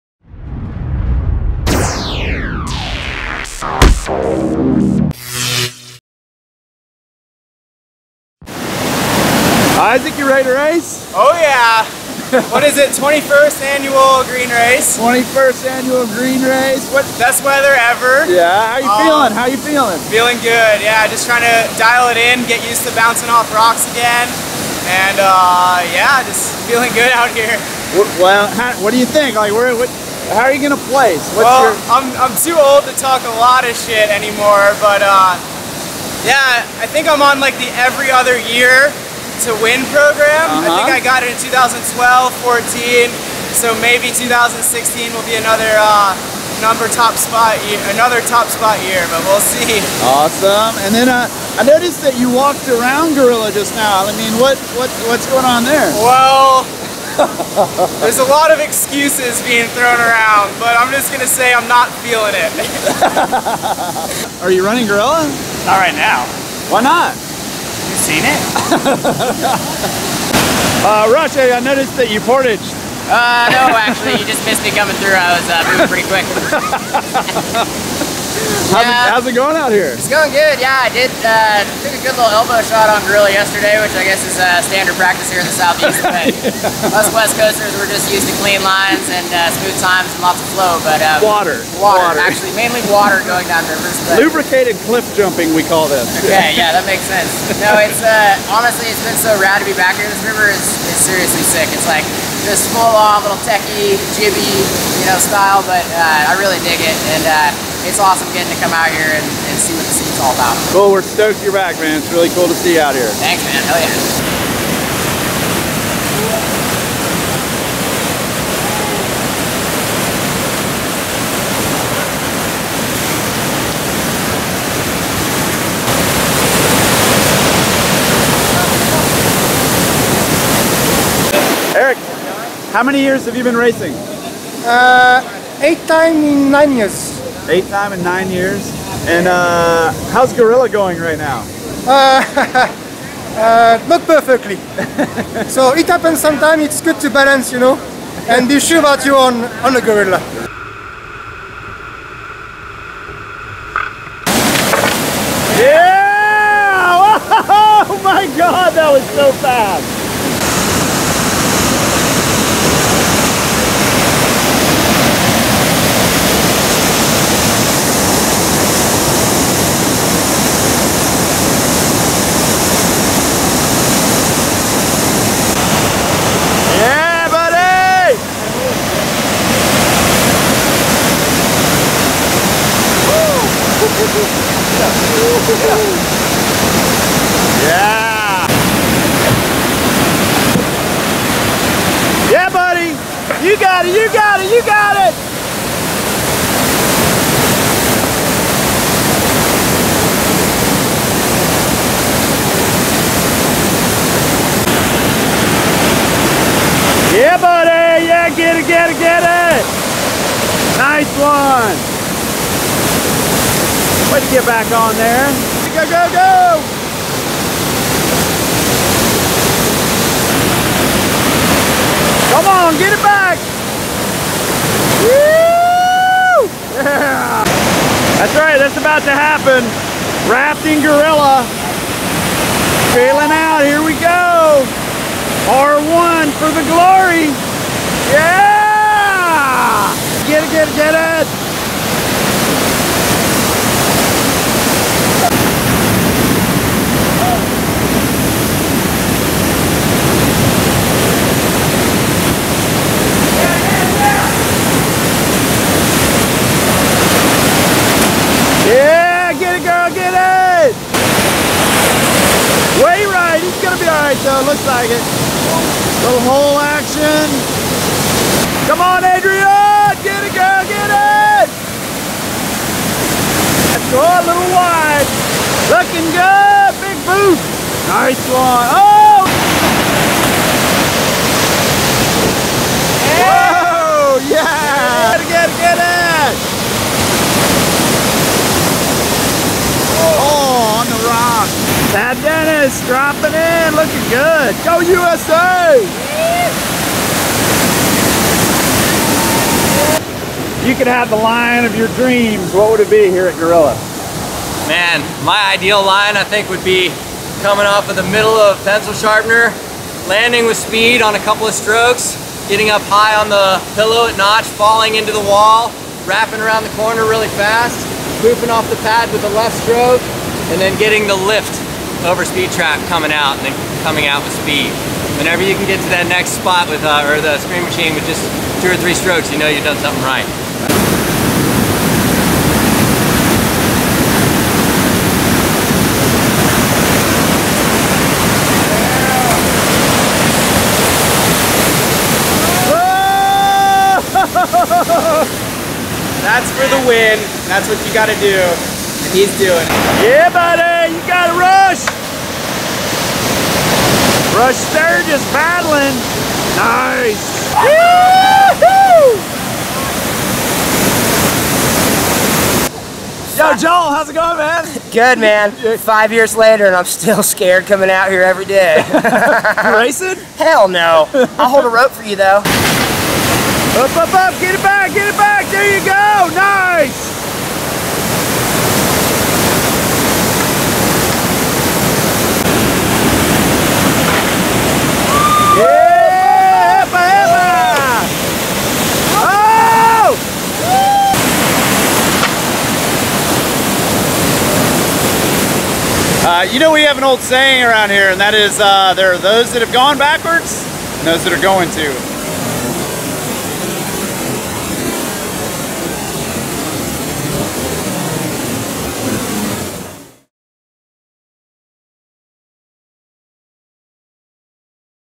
INSIDE the 2016 Green Race #2 – Sweet Lines and Carnage at Gorilla
Welcome to the 21st annual Green Race! Like every year whitewater legend Shane Benedict is covering the Annual Green Race for us at Kayak Session mag. Today Shane stood at the infamous Gorilla, the most iconic rapid of the run...Crazy water levels have made Gorilla an unpredictable beast! Here are some of the fast lines and characters that make the Green Race such an awesome event! And off course a few carnage runs for the viewers pleasure... stand by for more...